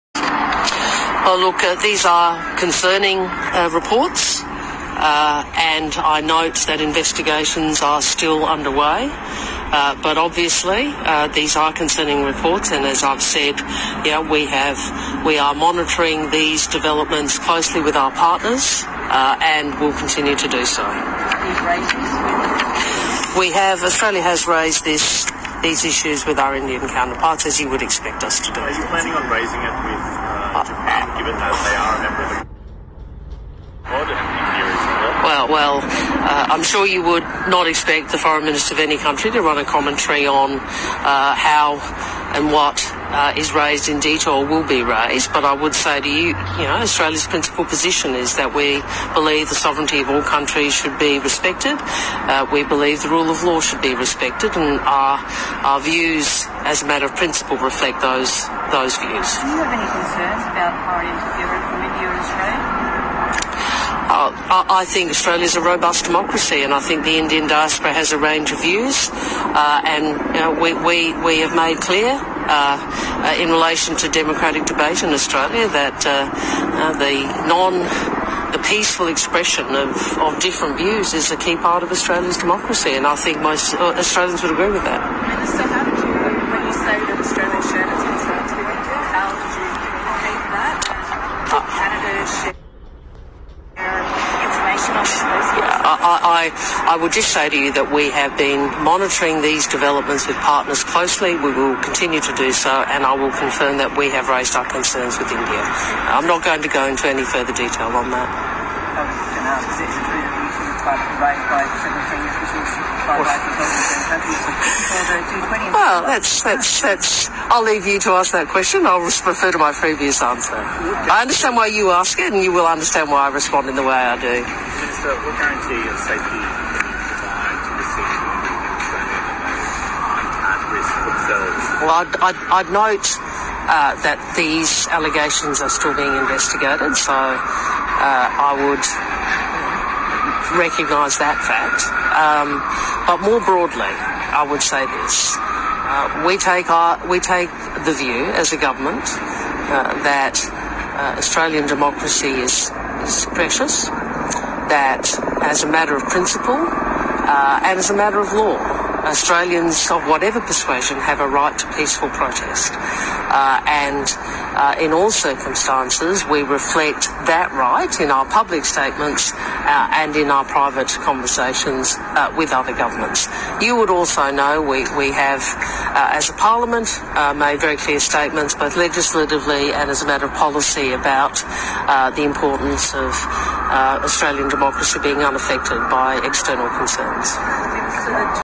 Australia Reacts To India Canada Diplomatic Row & Trudeau's Claims Over Nijjar's Killing
Calling the allegations raised in the Canadian Parliament about India’s alleged role in the killing of Khalistani leader Hardeep Singh Nijjar, as “concerning”, Australian Foreign Minister Penny Wong said that Canberra is closely monitoring the situation. Wong also noted that an investigation is underway into the matter, and Australia will continue to monitor the developments. “These are concerning reports. I note that investigations are still underway, but these are concerning reports. As I have said, we are monitoring these developments closely with our partners and we will continue to do so,” Wong said in a statement made at the sidelines of the United Nations General Assembly in New York.